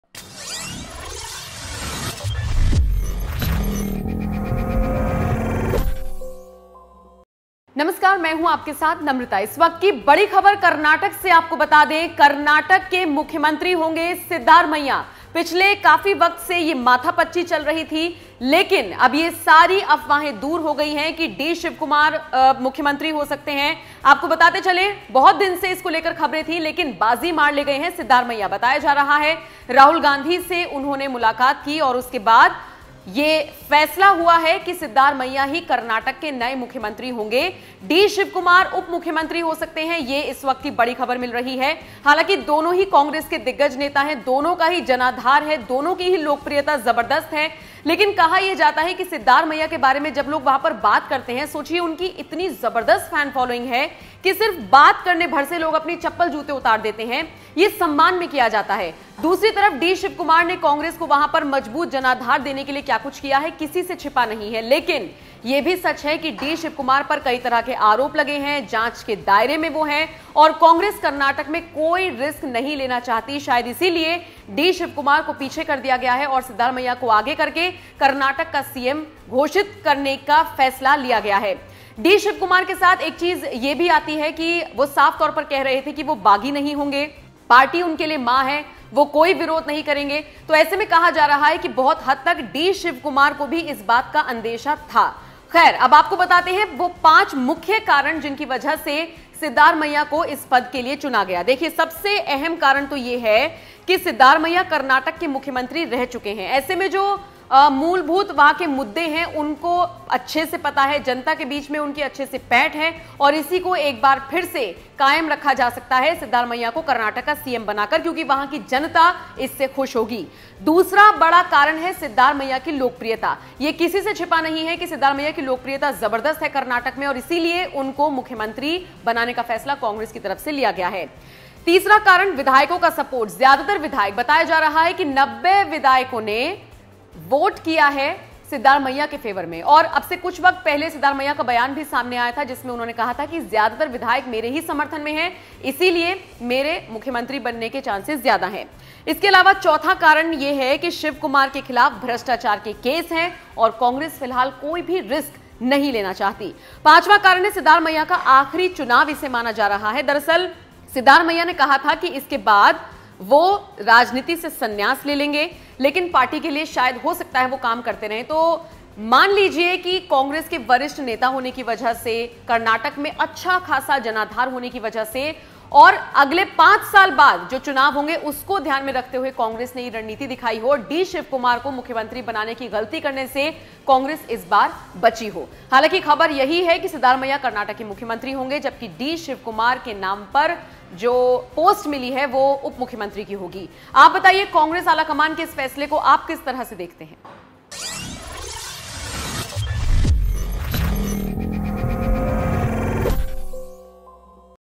कांग्रेस ने लिया सबसे बड़ा फैसला, कर्नाटक के सीएम होंगे सिद्धारमैया, 18 मई को लेंगे शपथ
कांग्रेस ने लिया सबसे बड़ा फैसला, कर्नाटक के सीएम होंगे सिद्धारमैया, 18 मई को लेंगे शपथ vinay
amrata